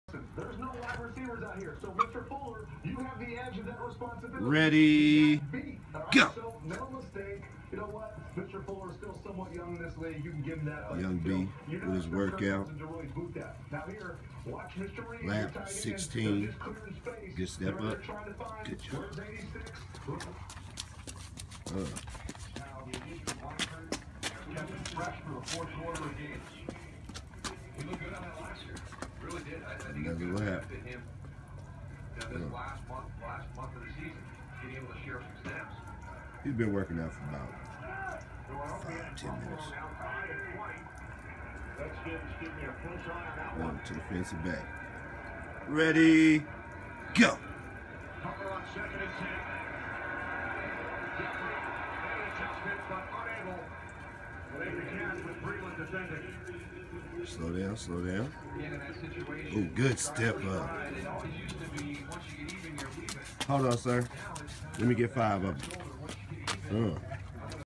Young B workout
Everyday he's working out already trying to be better then the rest. What kid u know does his own daily workouts?